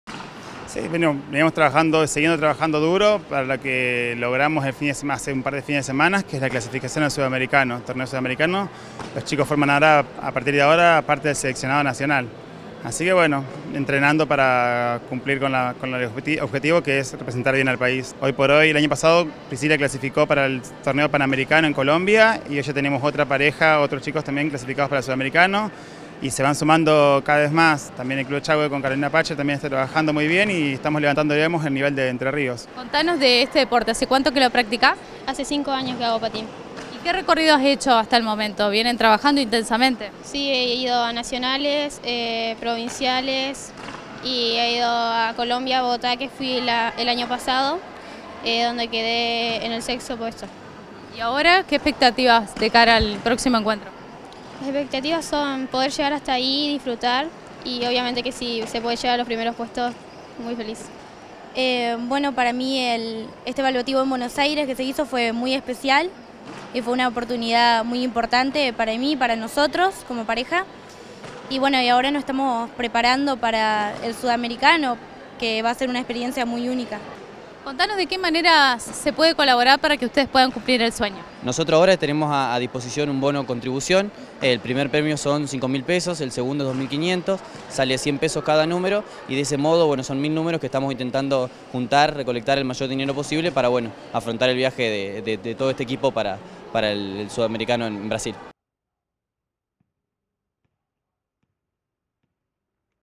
PATIN
TRES PATINADORES DEL CLUB SAN MARTÍN VIAJAN
AL SUDAMERICANO CON LA SELECCIÓN NACIONAL . PARA PODER AYUDARLOS LANZARON UN BONO CONTRIBUCIÓN
Leonardo Maidana
Priscila Iturbide
Victoria Rigo
Gustavo Dreiszigacker